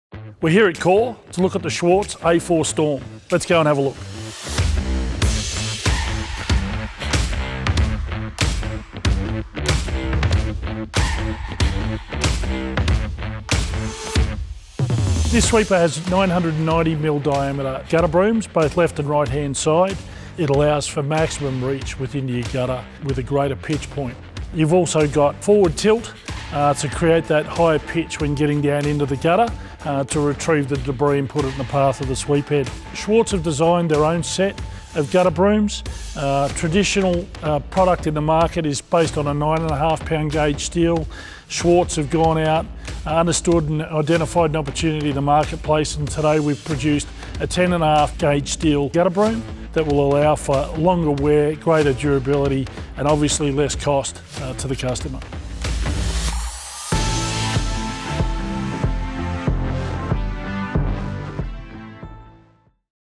Gutter Brooms of the Schwarze A4 Storm Regenerative Air Sweeper
Learn about the Schwarze A4 Storm gutter brooms, featuring 10.5 gauge steel bristles for greater durability and 10% more service volume.

0:00 Introduction
0:13 Schwarze-Designed Gutter Brooms
The 990mm diameter gutter sweeper brooms, on both sides of the truck, allow for maximum reach with a greater pitch point into the gutter. These gutter-cleaning brooms are adjustable to a forward tilt to create a higher pitch for the collection of debris.